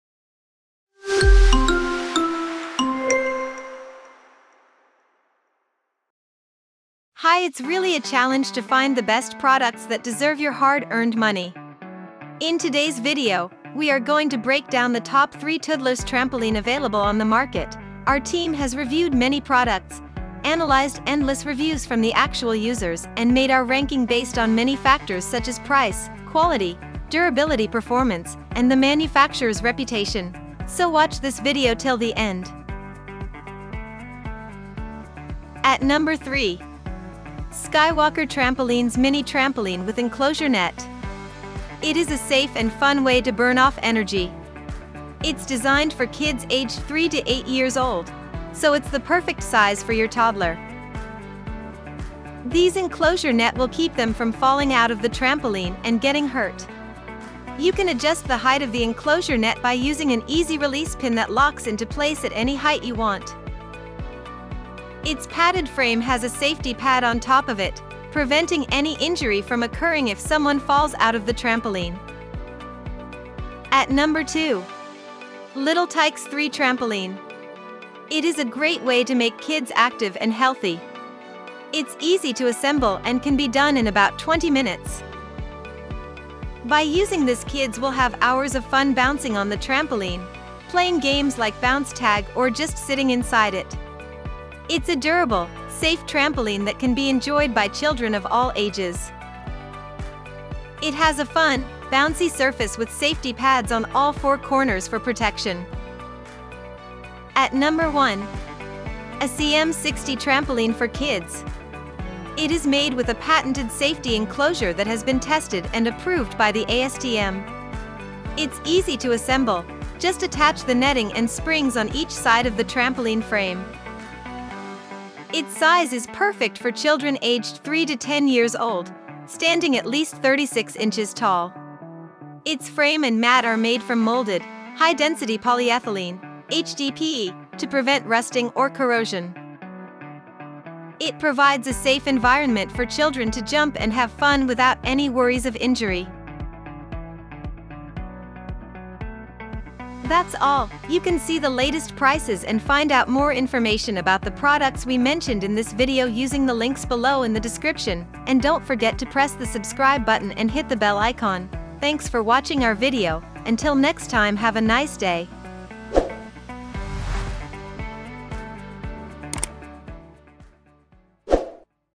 Best Toodlers Trampoline For Kids
Follow the links given below to check the product prices:

Asee'm 60" Trampoline for Kids:

Little Tikes 3' Trampolines:

Skywalker Trampolines Mini Trampoline with Enclosure Nets:
h [Amazon]

0:00​​ Best Toodlers Trampoline For Kids
0:35 Skywalker Trampolines Mini Trampoline with Enclosure Nets
1:13 Little Tikes 3' Trampolines
01:47 Asee'm 60" Trampoline for Kids
02:30 Closing Remarks & Subscribe To Our Channel